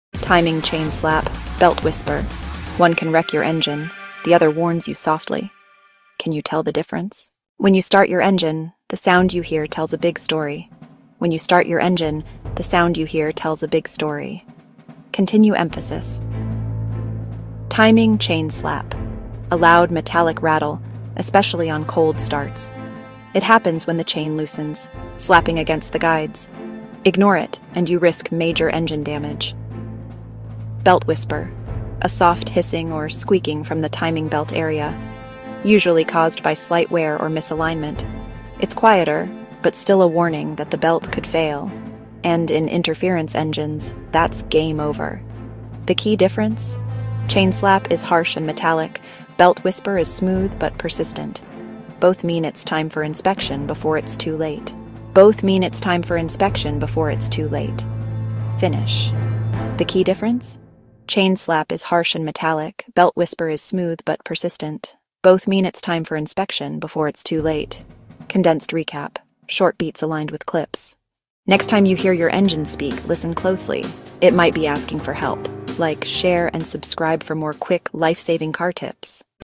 "Timing Chain Slap vs Belt Whisper – Can You Hear the Danger?"TimingChain
"🔧 Is that sound from your engine a timing chain slap or a timing belt whisper? In this quick video, we’ll explain how to identify these two noises, what they mean, and why ignoring them could cost you your engine.

⚙ Timing Chain Slap – Metallic rattle from a loose or worn timing chain hitting its guides.
⚙ Timing Belt Whisper – Soft hiss or squeak from a worn or misaligned timing belt.

Both are warning signs that your engine needs attention. Learn the difference, protect your car, and avoid costly breakdowns.

✅ Perfect for car owners, mechanics, and automotive enthusiasts.